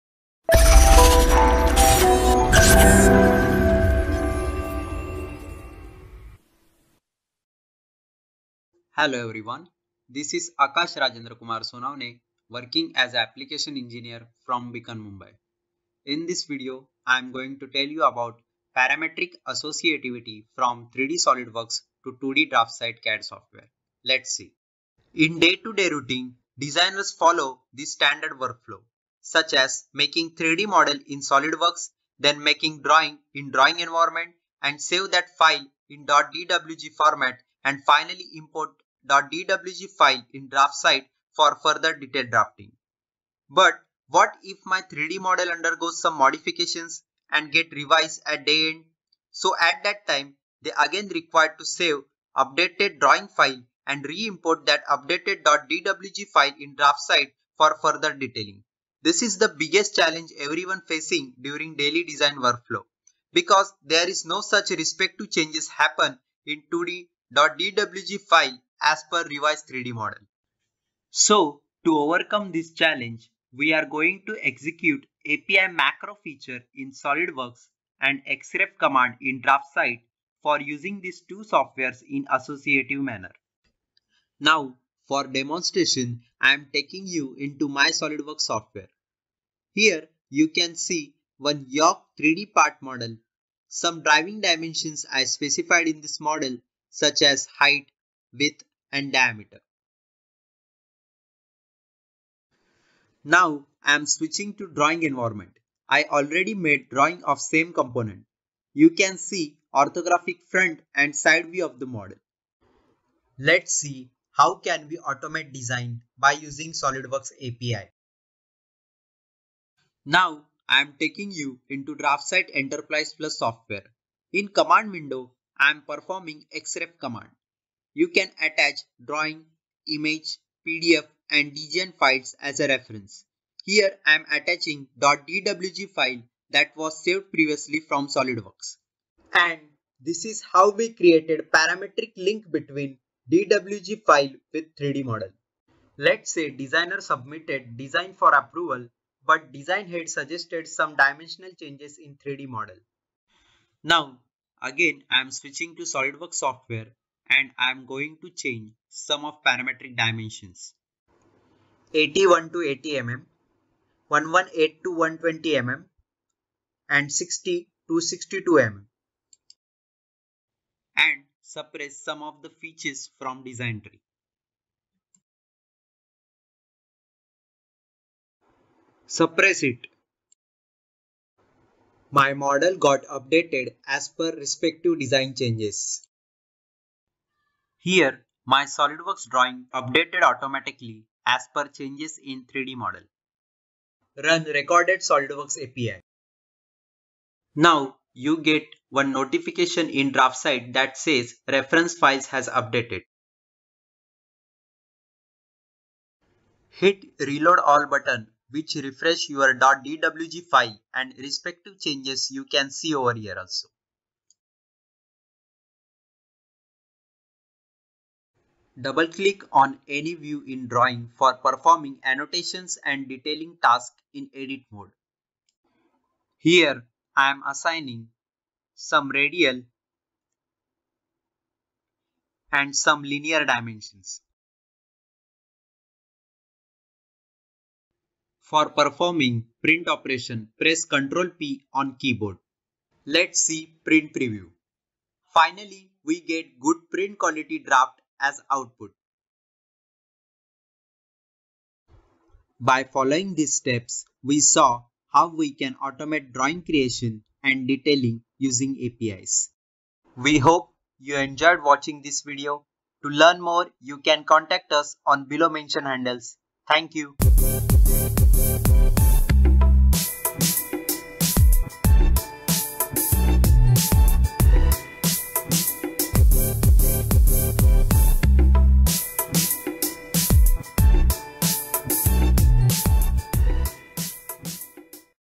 Parametric Associativity from 3D SOLIDWORKS to 2D DraftSight CAD Software | BEACON
In this video you can see how you can create Parametric associativity from 3D SOLIDWORKS to 2D DraftSight CAD Software by using SOLIDWORKS API with XREF Functionality in DraftSight by Mr. Aakash Sonavane, Application engineer, BEACON.

In day-to-day routine, Designers follows standard workflow, such as making 3D Model in SOLIDWORKS, then making drawing in drawing environment & save that file in .dwg format & finally import .dwg file in DraftSight for further detailed drafting.

But What if my 3D Model undergoes some modification & get revised at day end? My .dwg file remains as it is there is no changes happens as per modification in 3D Model. So to overcome this challenge, just follow this video...

For Demonstration or Enquiry regarding our Solutions, please get in touch with us